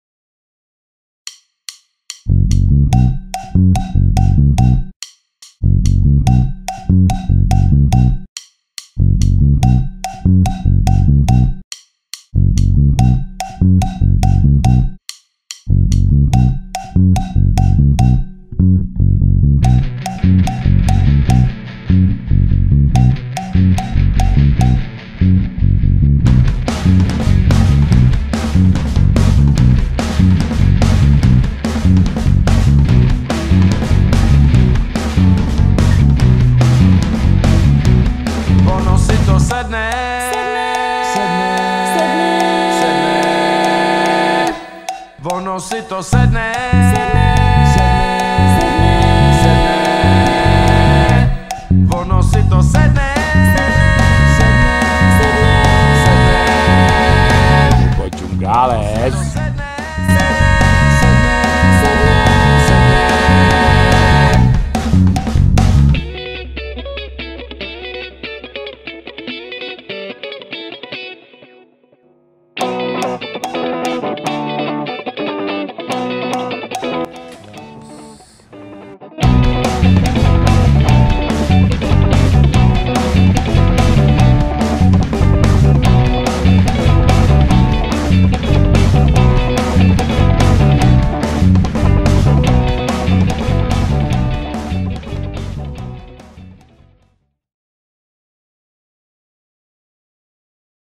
Wono Sito Sedne goes to Gama rádio...
Reportáž k rozhovoru v Gama rádiu ze dne 21. května 2014.